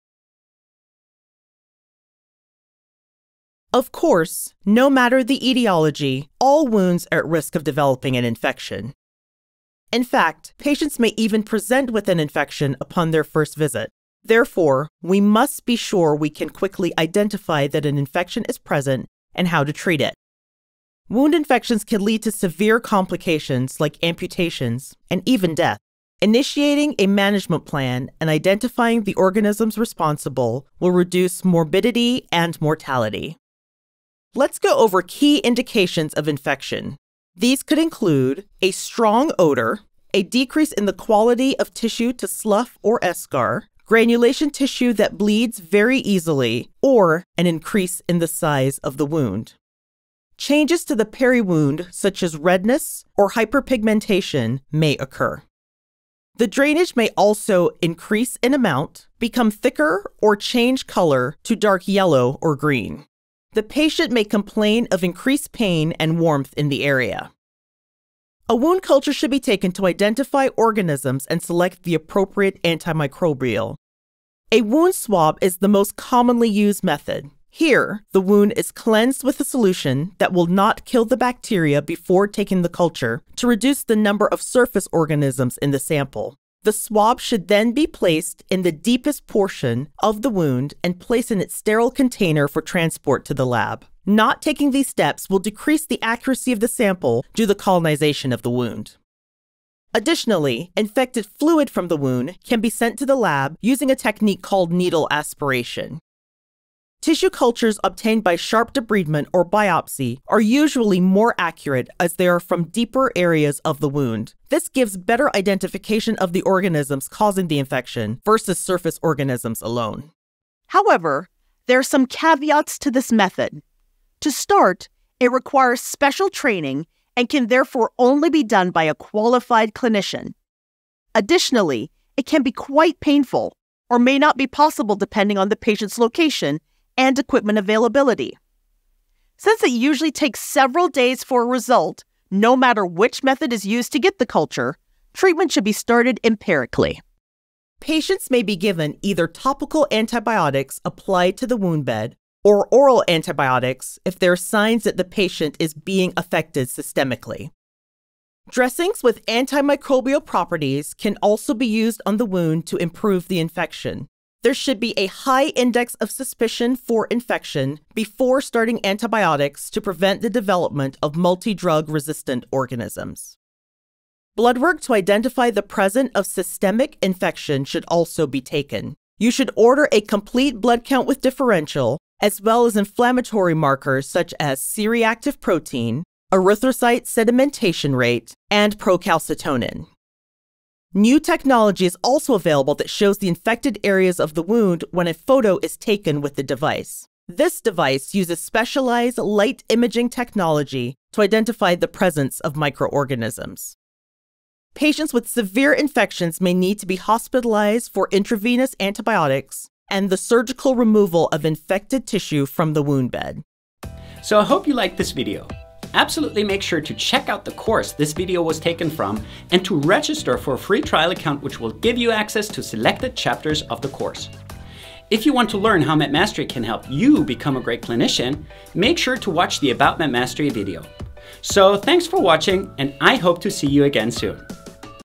How to Rapidly Identify and Treat Wound Infections (before they become deadly/life-threatening)
Ever faced a wound that just won’t heal? Learn how to identify and treat wound infections before they turn critical. This video arms you with the latest techniques and technologies to swiftly detect signs of infection so you can take immediate action and prevent complications like amputations (or worse).
Learn the art of accurate wound swabbing, with insights on common errors that could compromise your diagnosis as well as understanding when to prescribe antimicrobials so you reduce the likelihood of resistance. You’ll also learn about the groundbreaking imaging technology that makes diagnosis quicker than ever and reveals hidden signs of infection. If you’re a clinician who often finds yourself treating wounds, this video is for you!